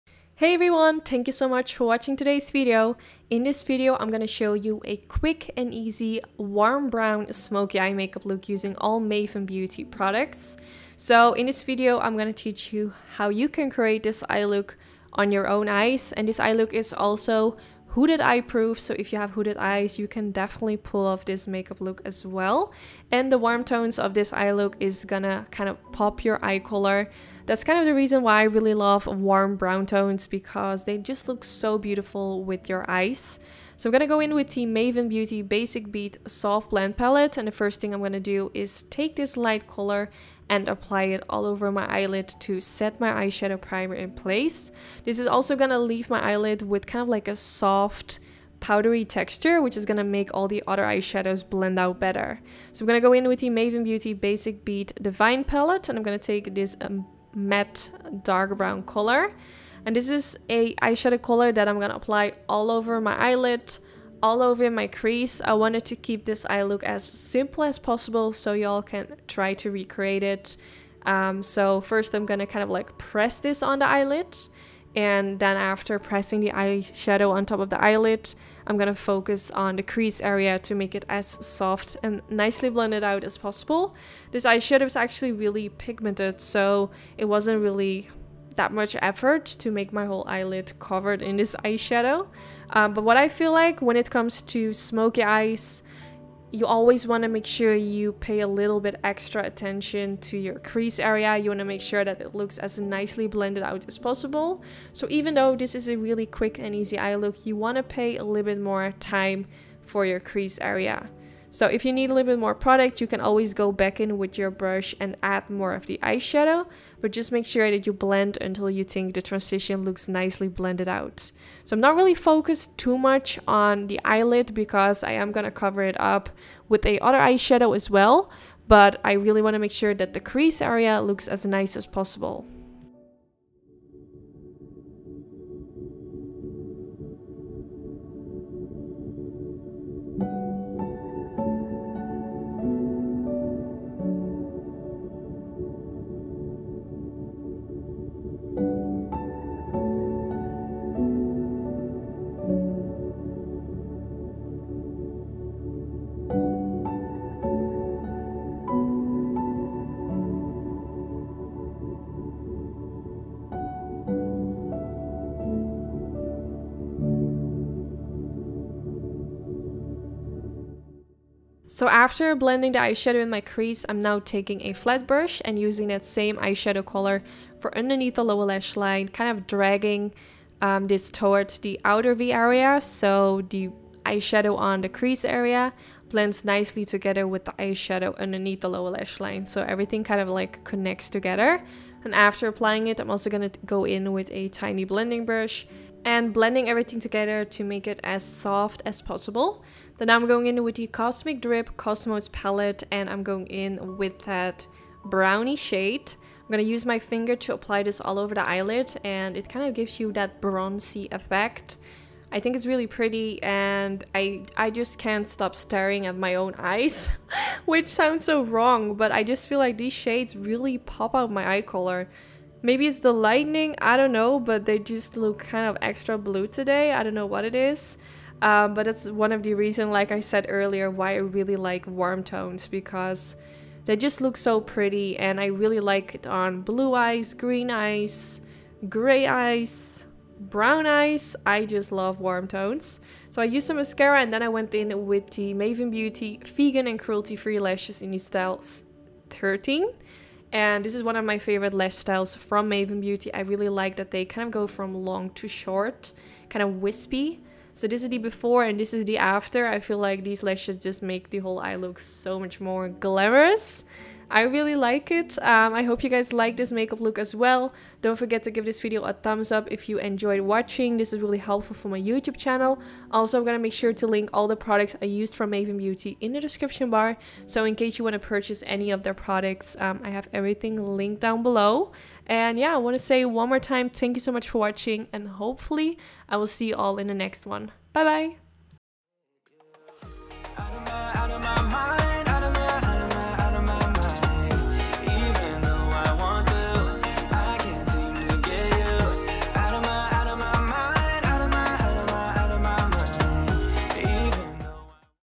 5 MINUTE Easy Brown Smokey Eye Tutorial | Eyelook for Hooded Eyes
In today's video I am showing you a 5 Minute Eye Makeup Tutorial in which I show you a brown smokey eye look which can be worn for people with Hooded Eyes as well! I hope you like how it turned out. Let me know if you're going to try it. This eyelook will also pop your EYES!! Can't wait to use more warm tones again this summer. It's my favorite to wear.

► PRODUCTS USED:

⇢ Cosmic Drip Eyeshadow palette in COSMOS
⇢ Maven Beauty Basic Beat Eyeshadow Palettes: Soft blend
⇢ Maven Beauty Cruelty Free Vegan Lashes in 13

► ABOUT